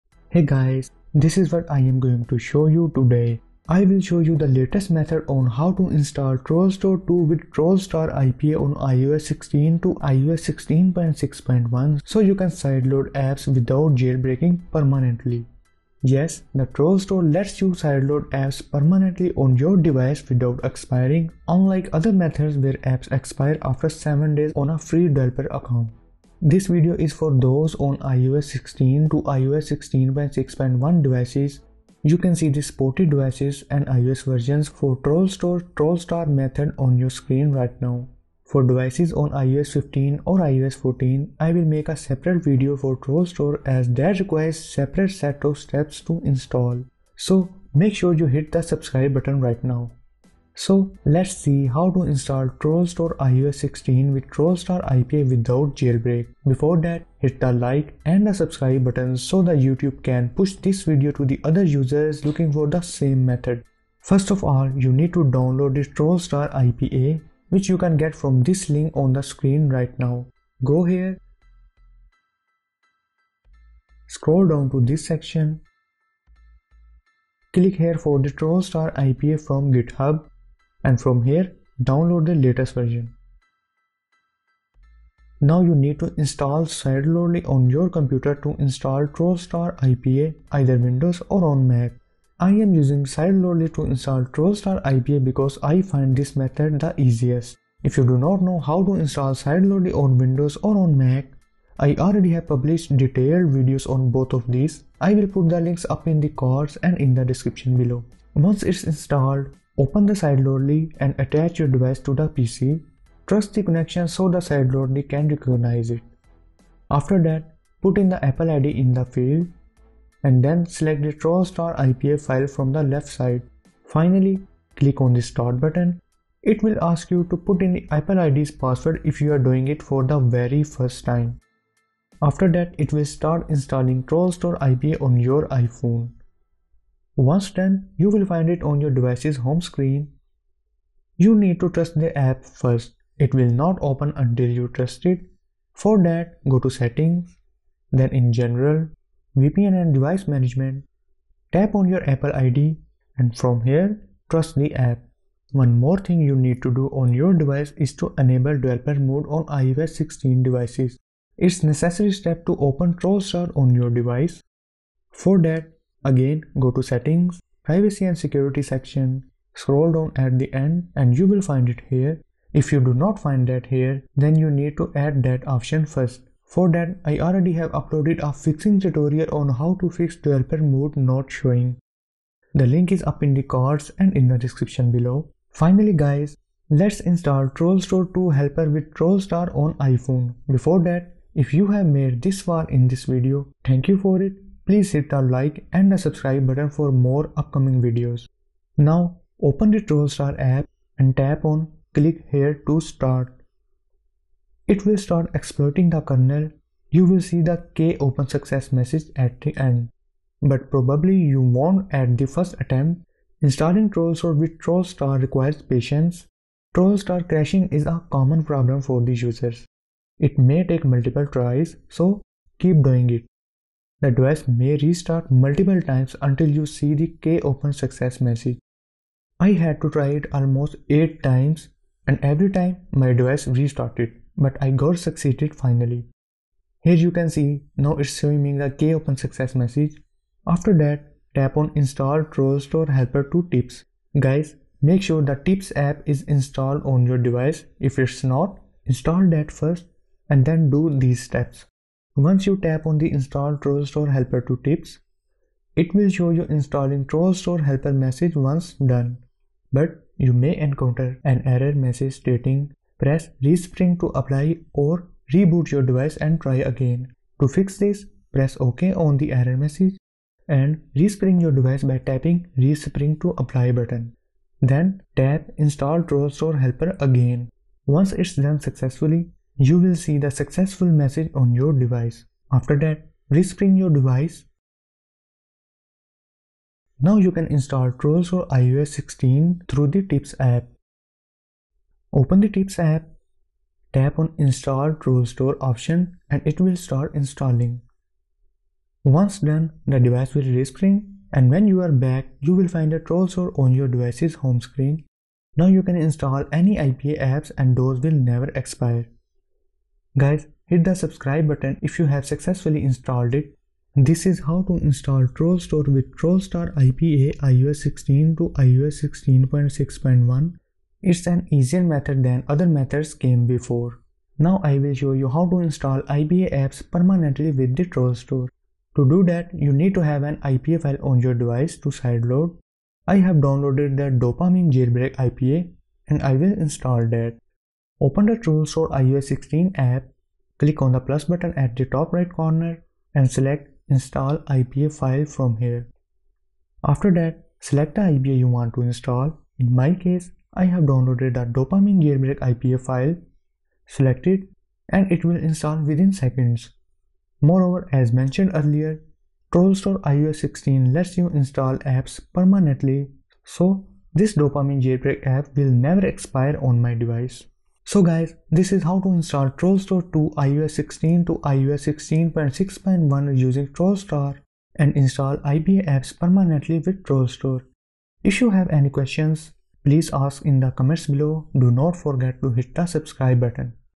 How to Install TrollStore 2 On iOS 16.0 - iOS 16.6.1 with TrollStar 2024
how to install trollstore 2 with Trollstar IPA on iOS 16 – 16.6.1 so you can sideload apps without jailbreaking permanently.

🔥🔥 *Useful Resources* 🔥🔥

Yes, the trollstore lets you sideload apps permanently on your device without expiring, unlike other methods where apps expire after seven days on a free developer account.
This video is for those on iOS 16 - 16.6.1 devices, you can see the supported devices and iOS versions for Trollstore Trollstar method on your screen. For devices on iOS 15 or 14, i'll make a separate video for trollstore as that requires separate set of steps to install, so make sure you hit the subscribe button.

So, let see how to install trollstore iOS 16 with Trollstar IPA without jailbreak. before that, hit the like and the subscribe button so the youtube pushes this video to other users looking for the same method.
first you need to download Trollstar IPA, which you can get from this link on the screen right now. Go here, scroll down to this section, click here for the trollstar ipa from github, and from here, download the latest version.
Now you need to install sideloadly on your computer to install Trollstar IPA either Windows or MAC; I am using sideloadly to install Trollstar Github because I find this method easiest.
if you dont know how to install sideloadly on windows or mac, i already have published detailed videos on both of these, ii'll put the links up in the cards, and in the description below.
once installed, Open the sideloadly, attach your device to the PC and trust the connection so that the sideloadly can recognize it.
Moreover, you also need to do on your device is to enable developer mode on the iOS 16 device. It’s a necessary step to open Trollstar on your device.
Now you’ll be able to open the Trollstar app, open it, and it has instructed all the steps.
For that, go to settings - privacy and security - scroll to the end, and you’ll find it here. If you don’t find that here, then you need to add that option, for that i have uploaded a fixing tutorial on how to fix developer mode not showing, the link is up in the cards and in the description below.
Finally, Let's install trollstore helper 2 with Trollstar on iphone.

*TimeStamps*

0:00 Intro
0:31 Supported Devices for Trollstore Trollstar
1:03 Download Trollstar IPA Github
1:25 Install Sideloadly on Windows / Mac
1:48 Sideload / Install Trollstar IPA iOS 16
2:20 Trust the app and enable Developer mode
3:18 Install Trollstore Helper to tips with Trollstar app
4:51 Install Trollstore 2 from Tips app
5:23 Install Apps with Trollstore 2